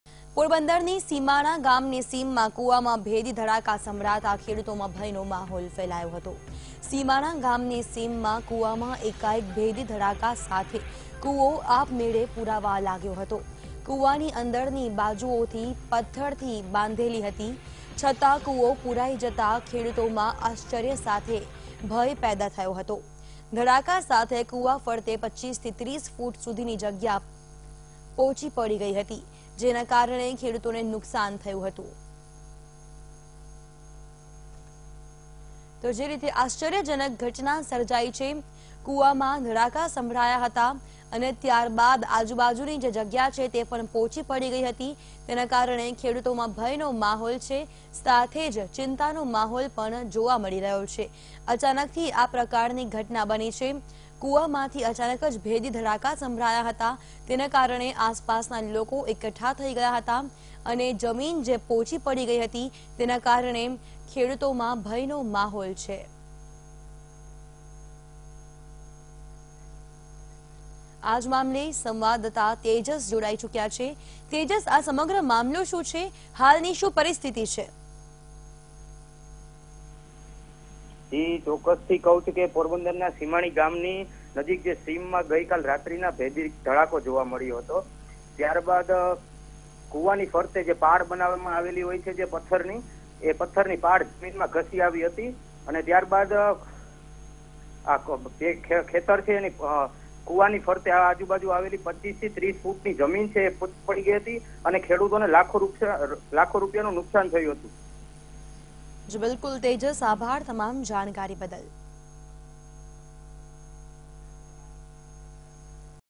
Porbandar: Enigmatic explosion happened in the farm pit in Simani village | Vtv News
Porbandar: Enigmatic explosion happened in the farm pit in Simani  village